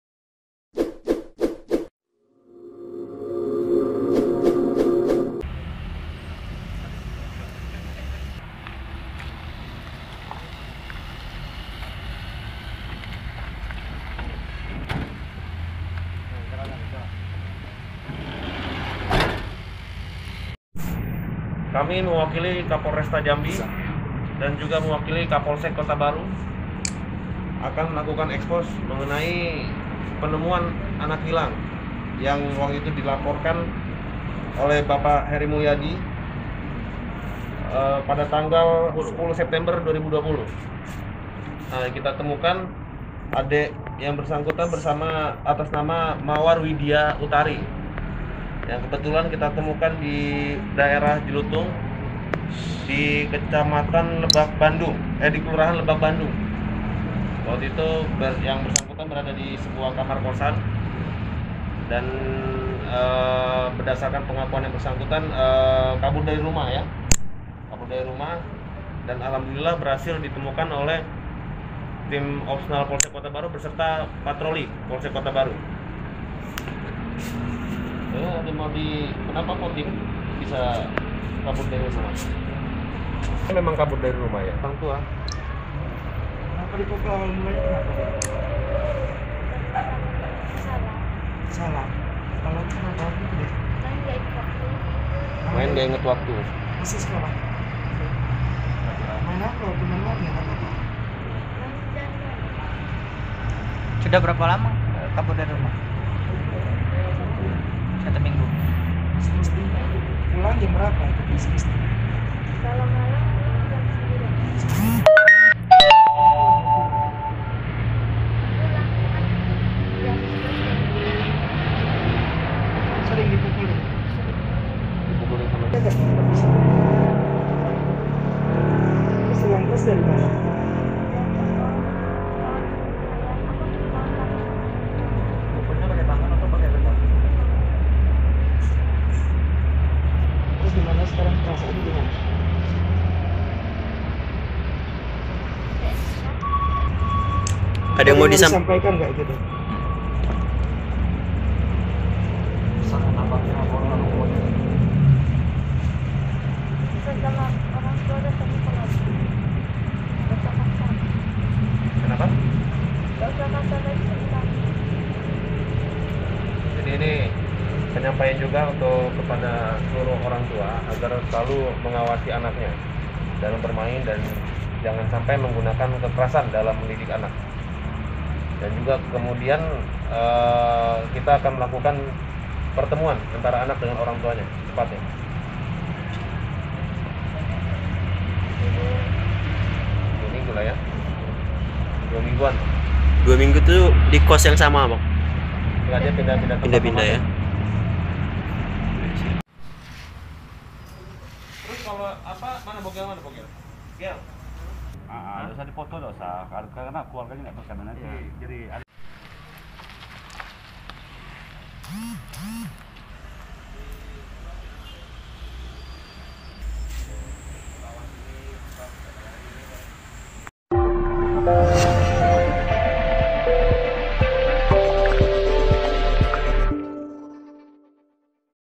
Mendapat Kekerasan Dari Keluarga, ABG 16 Tahun Di Kota Jambi Pilih Kabur Dari Rumah
Laporan Wartawan Tribun Jambi, Aryo Tondang

TRIBUNJAMBI.COM,JAMBI- Sempat dilaporkan hilang, MW, seorang wanita remaja usia 16 tahun di kota Jambi ini berhasil ditemukan oleh Tim Opsnal Reskrim Polsek Kotabaru.

MW ditemukan di sebuah kamar kos yang berada di kawasan Lebak Bandung, Jelutung, Kota Jambi, Rabu (28/10/2020) pukul 16.00 WIB.

MW dilaporkan oleh orang tuanya ke Polsek Kotabaru, pada pada (27/10/2020).

Saat itu, orang tua korban mengaku, MW pergi dari rumah sejak Kamis (10/9/2020) pukul 15.00 WIB, saat itu MW pergi bersama satu orang temannya, bernama Karin.

Sekira kurang lebih 1 bulan melarikan diri, MW akhirnya berhasil ditemukan.

Kepada sejumlah awak media, MW mengaku nekat pergi dari rumah, lantaran sering dimarahi oleh orang tua dan kakak kandungnya sendiri.

Tidak hanya menerima bentakan, dirinya juga kerap mendapat perlakuan keras, hingga pukulan dari kakak dan ayah kandungnya tersebut.

"Sering dipukul saya, karena sering pulang jam 10 malam," kata MW, Rabu (29/10/2020) malam.

Saat memutuskan untuk pergi dari rumah, MW hanya mengandalkan pertolongan dari teman-temannya, dan mencari tempat menginap secara berpindah-pindah.

Saat berhasil ditemukan dan dibawa ke Polsek Kotabaru, kondisi MW tampak sedikit memprihatinkan.

MW yang sempat dikabarkan hilang tersebut, tampak lusuh, hanya dengan satu pasang baju warna hitam, celana panjang warna abu-abu dan sendal jepit.

Usai berhasil ditemukan sejak menghilang 1 bulan lalu, MW meminta agar orang tua dan kakaknya tidak bertindak kasar kepada dirinya.

"Orang tua jangan main pukul lagi, jangan kasar-kasar," paparnya.

Sementara itu, Kapolsek Kotabaru, AKP Afrito Marbaro Macan, melalui Kanit Reskrim, Ipda Rizki M Ramadhan mengatakan, MW di temukan disebuah rumah kos, dengan kondisi selamat, tanpa adanya tanda kekerasan fisik.

"Kita terima laporannya dan langsung melakukan pencarian, hingga berhasil ditemukan," kata Rizki, Kamis (29/10/2020) malam.

Dalam kesempatan tersebut, Rizki mengimbau, agar para orang tua tidak melakukan kekerasan dalam mendidik anak dan lebih teliti dalam melakukan pengawasan terhadap anak.

Untuk MW, yang sempat dilaporkan hilang tersebut, kata Rizki akan segera dikembalikan dan diserahkan kepada pihak keluarga.

"Kita akan langsung pertemukan antara anak dan orang tuanya," tutup Rizki.